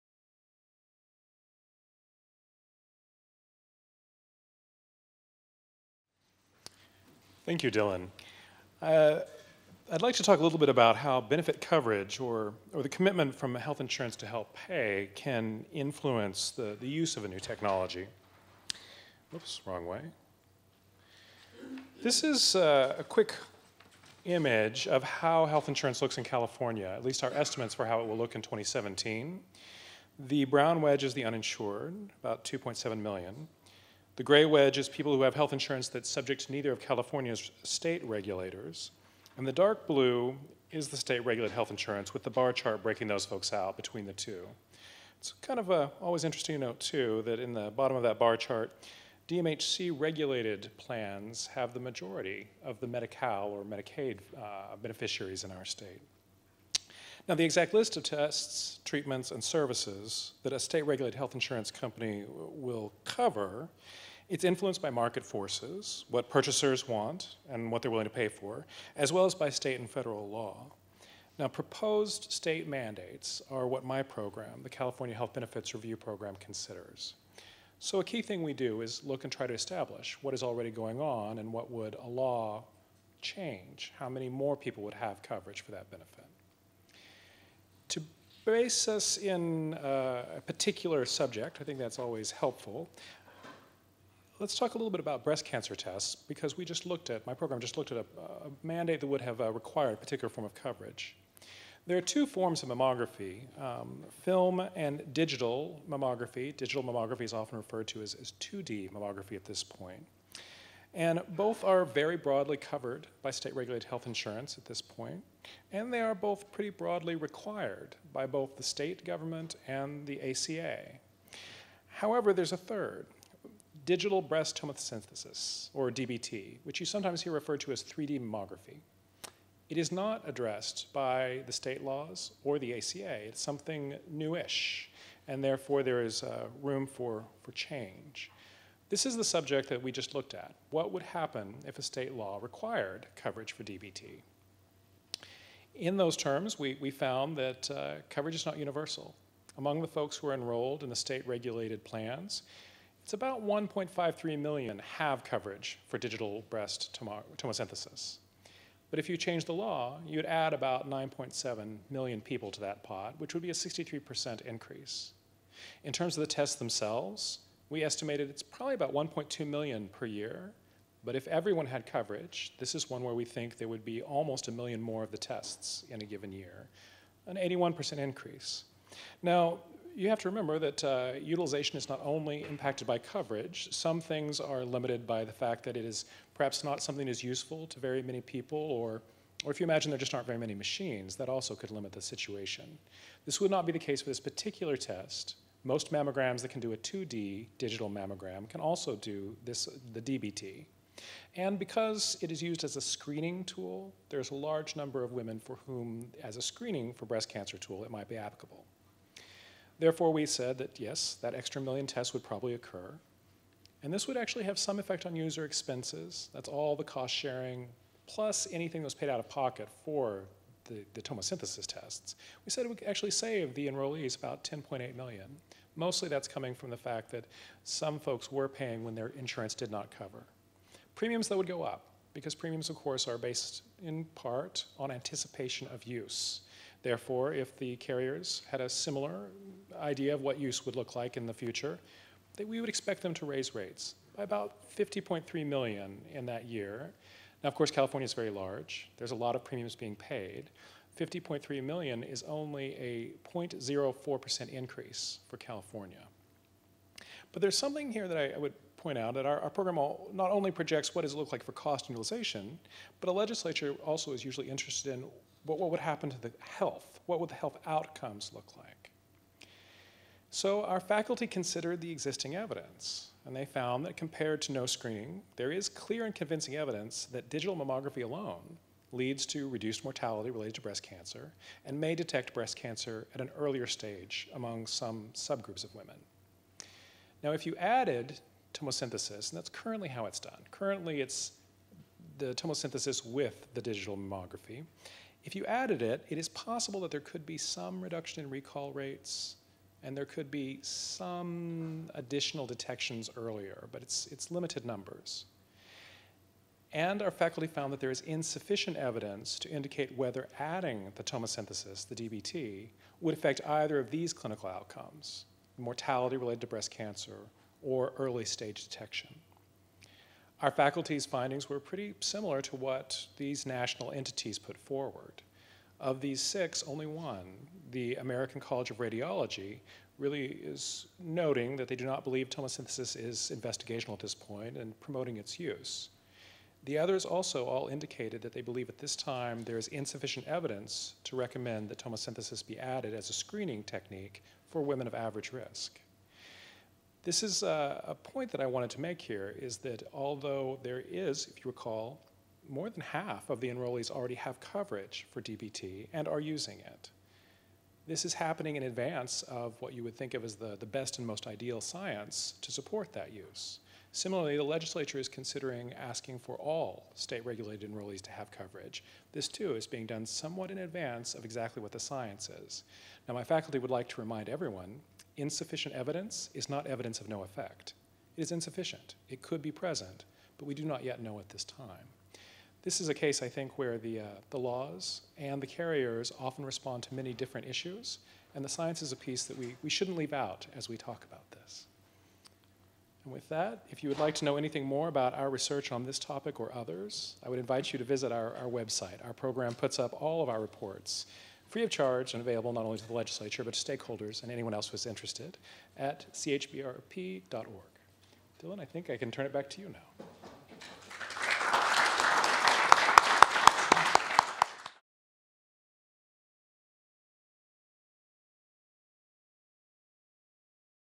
John Lewis – Opportunities and Barriers to Innovation and Technology after the ACA
The Paul Torrens Health Forum at the UCLA Fielding School of Public Health

"Opportunities and Barriers to Innovation and Technology after the ACA"

John Lewis, MPA
Associate Director
California Health Benefits Review Program

The Affordable Care Act has resulted in new insurance coverage for millions of Americans, as well as more generous benefits for many of those previously insured. However, providers and patients face a multitude of decisions that vary by insurance payer, product, and government program. In addition to new covered benefits and alternative reimbursement mechanisms, the health system could benefit from opportunities for innovation. In this Health Forum, our speakers will explore how technology and innovative models of care could expand through insurance coverage and reimbursement, while also understanding what limits –– in terms of payment, coverage, and incentives –– might act as a barrier to patients' and providers' access to services.